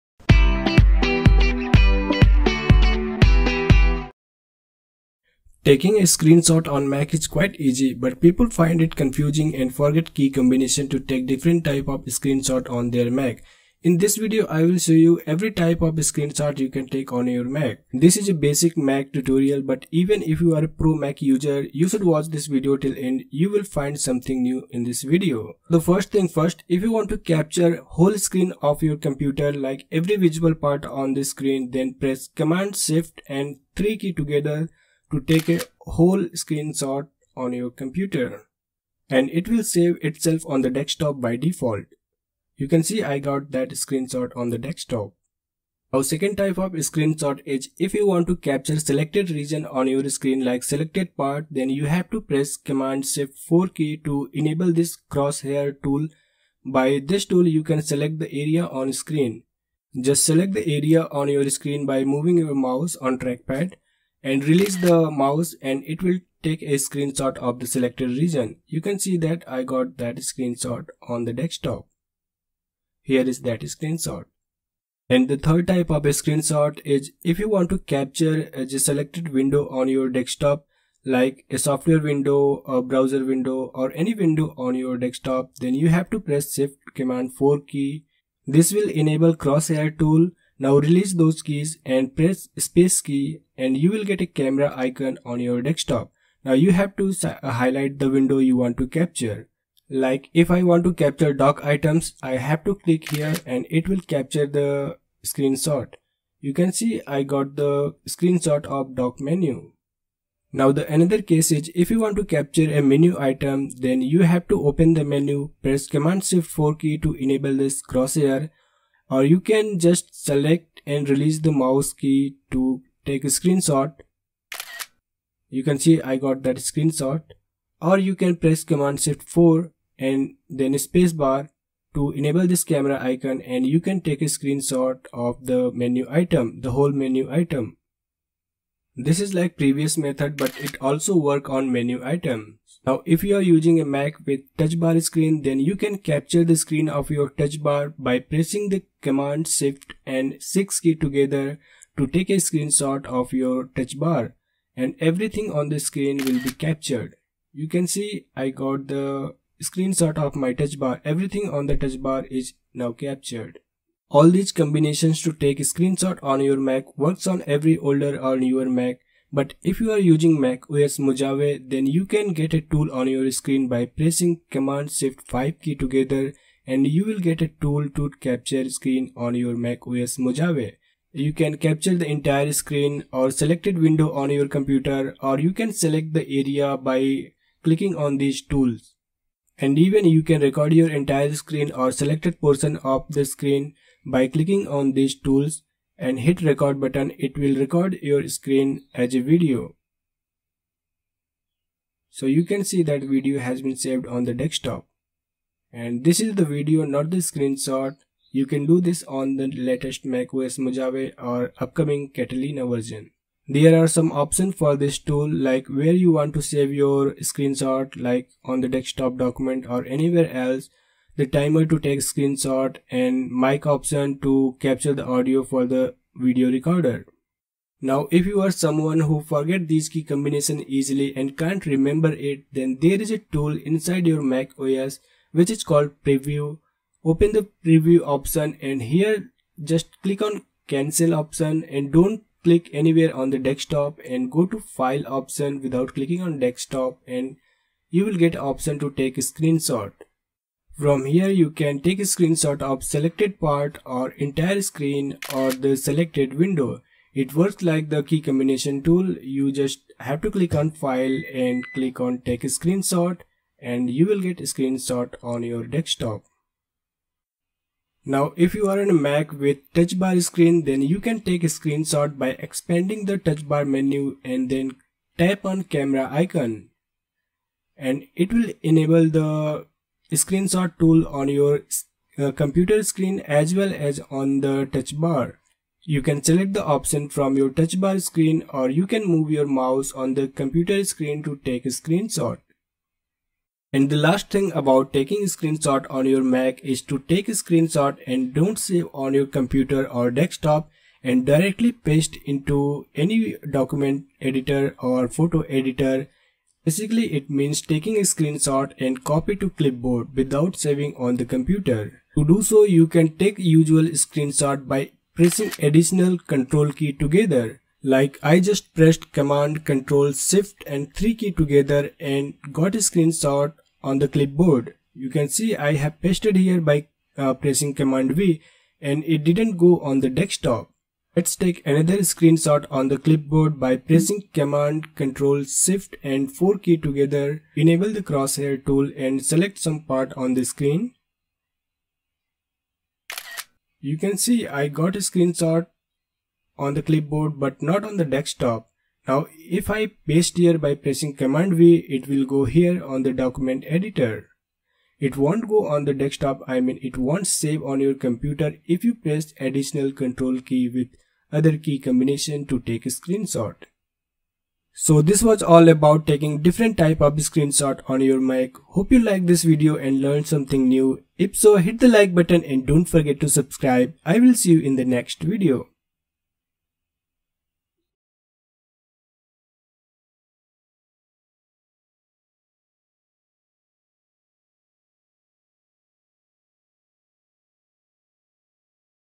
How to Screenshot Macbook Screen | Mac Basics
How to take a screenshot on Macbook Air, Pro, or any Mac OS easily? Taking screenshots on mac is easy but people find it confusing and can't remember the key combinations. There are different types of screenshots you can take on your Macbook Air, Pro, or Mac Os on iMac, Mac Pro.

In this video, I have explained all types of screenshots and how you can capture them.

So here is all the Key combination which does you can use to take a screenshot on your Mac whether you have Mac OS Big Sur, Catalina, Sierra, Mojave, High Sierra, Yosemite, or any other mac OS.

This works on Mac computers, even if you have Macbook Air, Macbook Pro, or iMac it works on all devices.

1. Take a full Screenshot - To capture the entire screen on your Mac, press *Command + Shif + 3* keys together and it will capture a screenshot and save it on the desktop by default.

2. Capture Selected Regions - Press Command + Shift + 4 to enable the crosshair tool and select the area you want to capture.

3. Capture Selected Window- Press *Command + Shift + 4* to enable crosshair tool and then release those keys and not press the Space bar to enable camera icon, highlight the window you want to capture and click to take a screenshot.

4. Capture Menu items - Open the menu you want to capture and use the 3rd Method to take a screenshot of the Menu.

5. Screenshot of Touch bar - If you are using Macbook Pro with a touch bar then you can capture the touch bar screen by pressing *Command +Shift + 6* keys together.

6. If you are on macOS Mojave then you can get screenshot tools on your screen by pressing *Command + Shift + 5* keys together.

7. If you are someone who forgets key combinations easily then you can use *Preview" tool on your mac to take the screenshot.

8. If you want to capture a screenshot and copy it to the clipboard without saving it on the computer then you have to press the additional *Control* Key.

Eg- Command + Ctrl + Shift + 3

to take a full screenshot and copy to clipboard and you can paste it to any document editor or photo editor by pressing Command + V keys together.